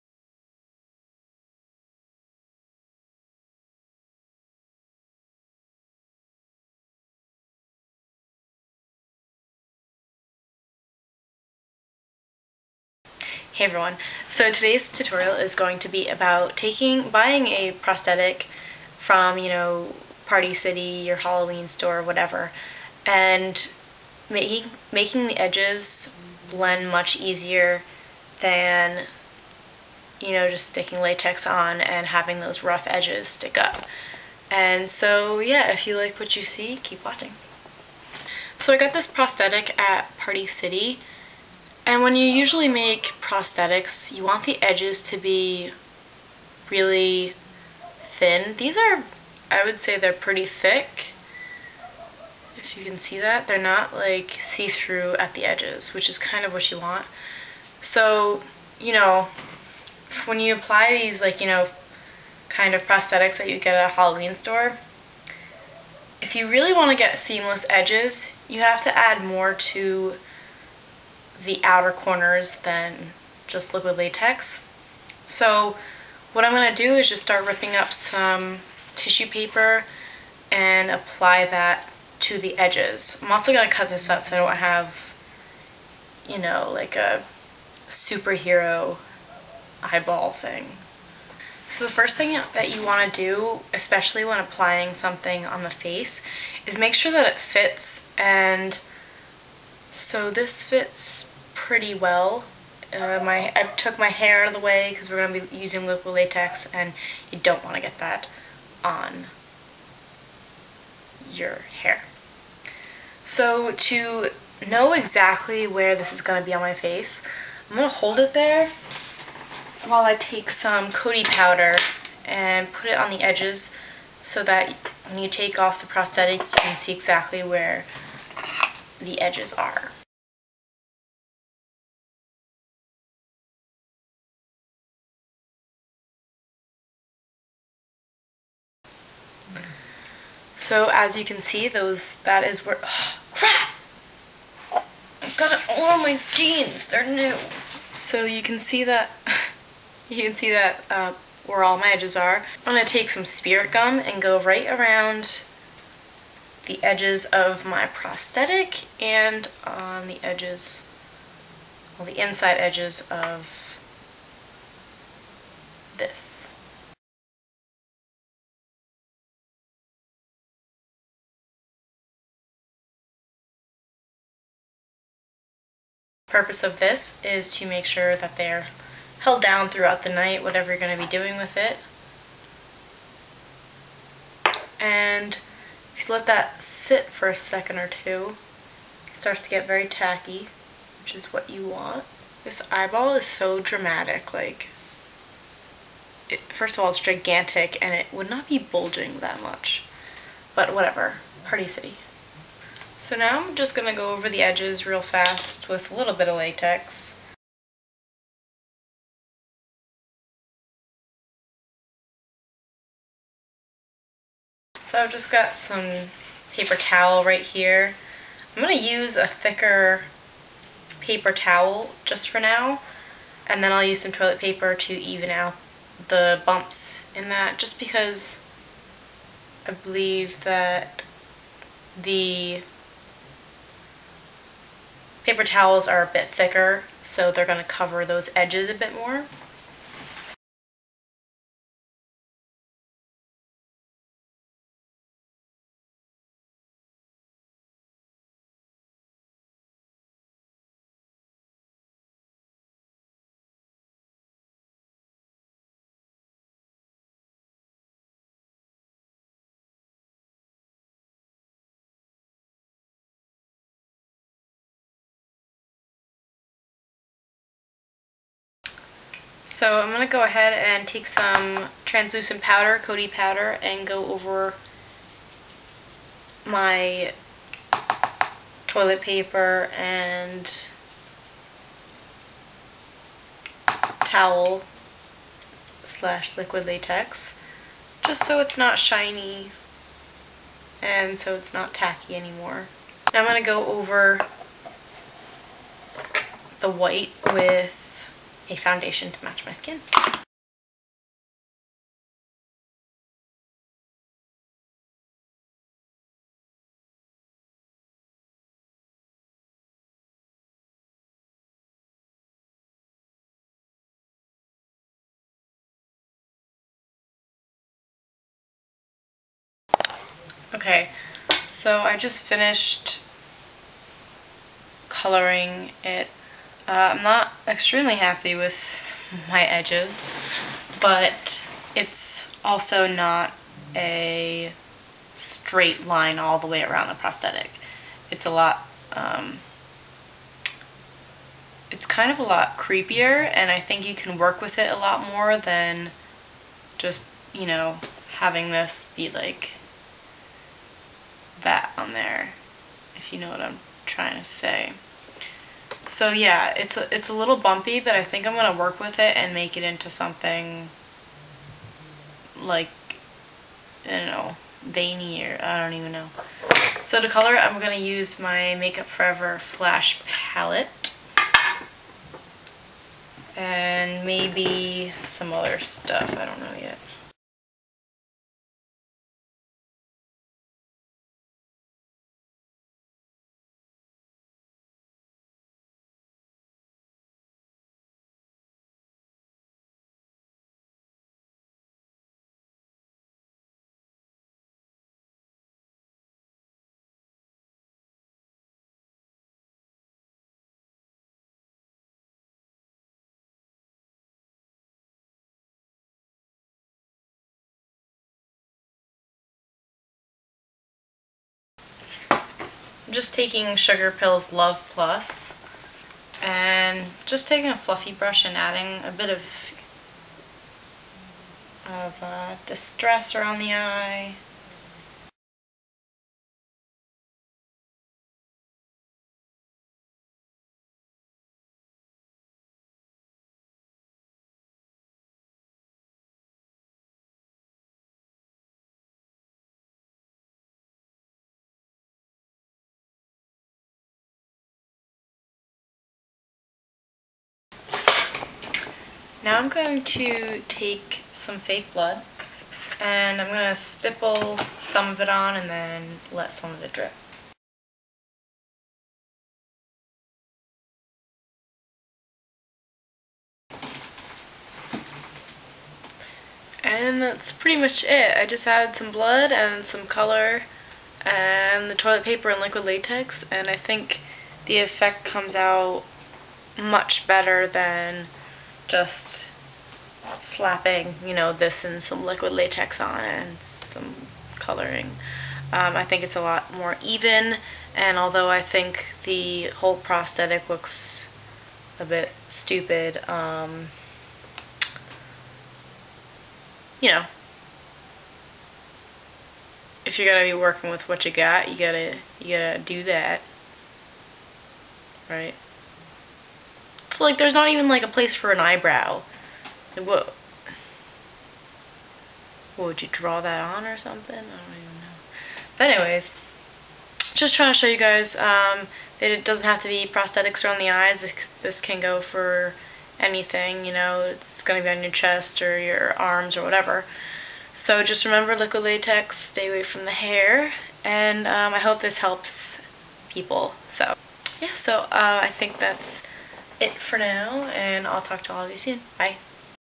How to: Making Bought Prosthetic Edges Better
Hope you all enjoyed!!  Yes I am aware that the edges are not invisble, however I think it looks much better than being able to see the original edges of the prosthetic!